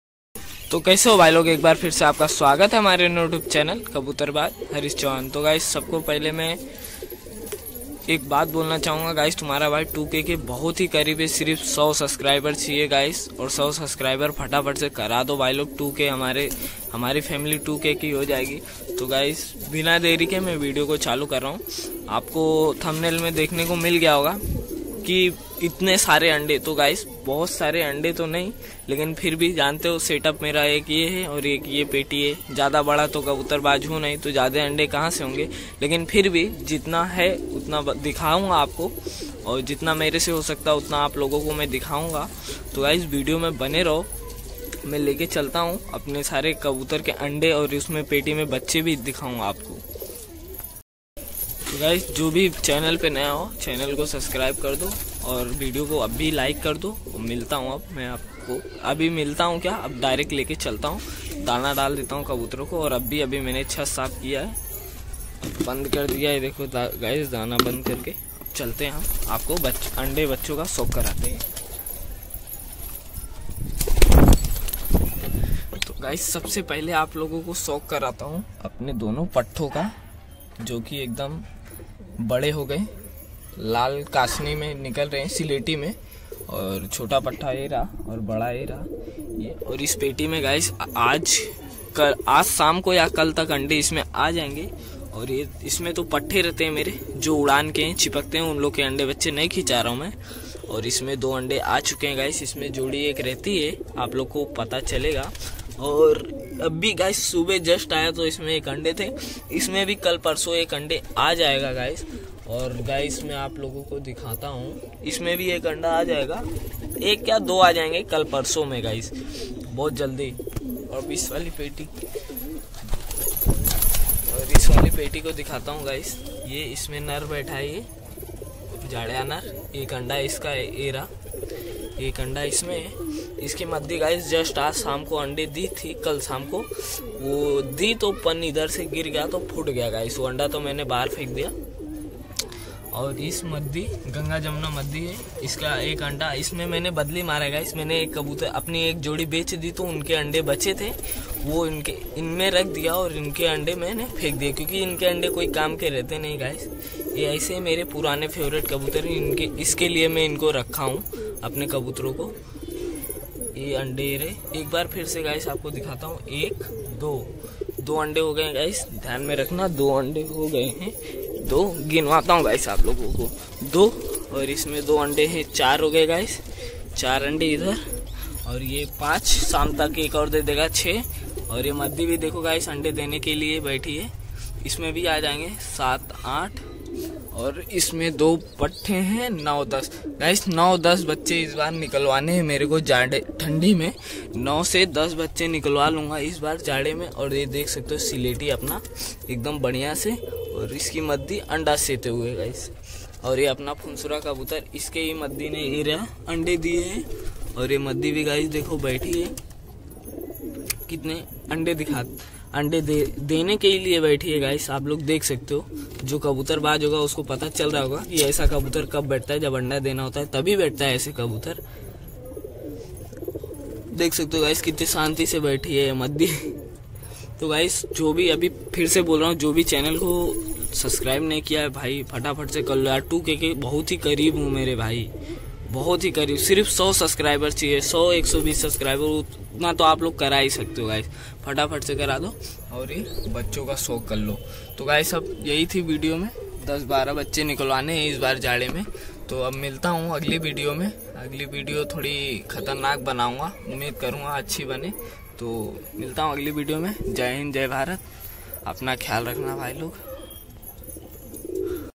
इतने सारे कबुतरो के अंडे 😯
es video mai hamne apna Briding setup ko dikhaya hai maine aur aap logo ko bhut sare ande to nhi fir bhe jitna hai utna dikhaya hai hamne video ko like karo 😇